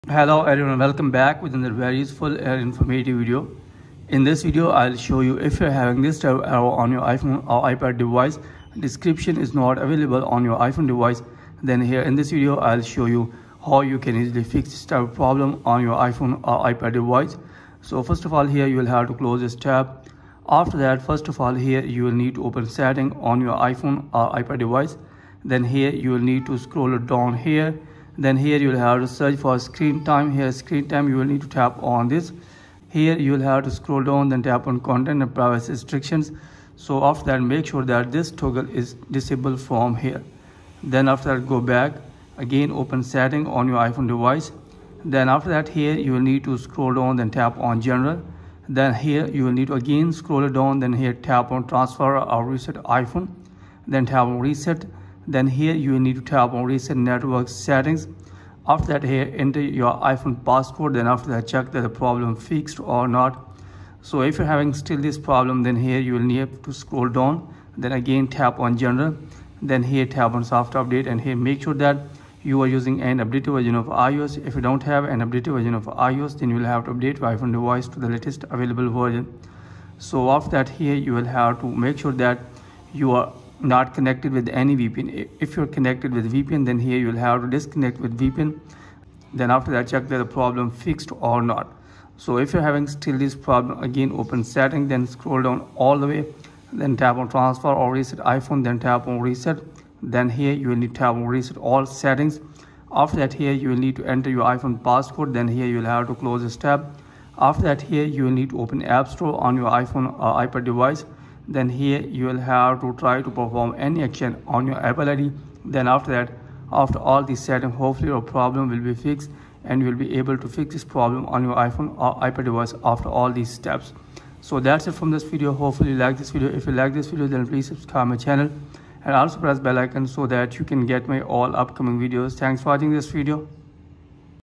Fixed✅ Apple ID error Description is not available | Verification Failed Apple ID
If you’re encountering the “Apple ID Error: Description is Not Available” message, it typically means there’s an issue with your Apple ID that cannot be fully explained by the system. This could happen due to several reasons, such as a temporary server problem, network connectivity issues, or an internal error in Apple’s system. Users may experience this error when trying to sign in to their Apple ID, access iCloud services, or perform tasks that require Apple ID authentication. It’s recommended to check your internet connection, try signing in again later, or contact Apple Support if the issue persist

How to fix error description not available apple?
Why are my Apple ID details unavailable?
Why is it saying my Apple ID is not available?
Why am I not able to create an Apple ID?

Timestamps:

0:00-Intro
0:10-How to fix error description not available apple
1:05-Apple ID Error Description not available